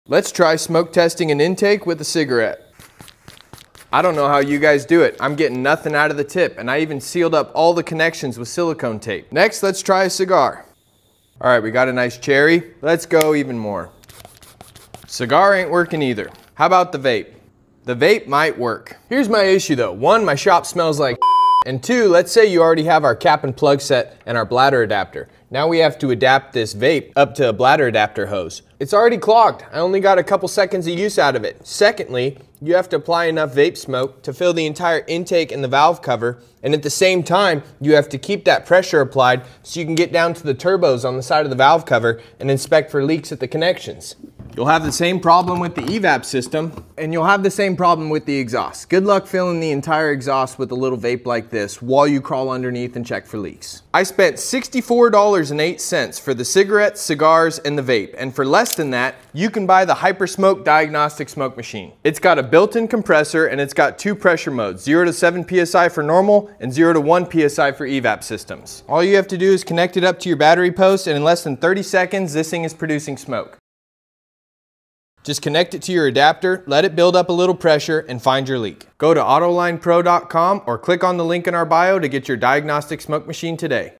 DIY Smoke Test | Diagnose Vacuum Leak, EVAP Leak, Boost Leak + More Fast Easy and Cheap
Trying to DIY smoke test with a vape? Good luck filling your exhaust system with that. HyperSmoke makes real diagnostic smoke in under 30 seconds, with a built-in compressor and dual pressure modes.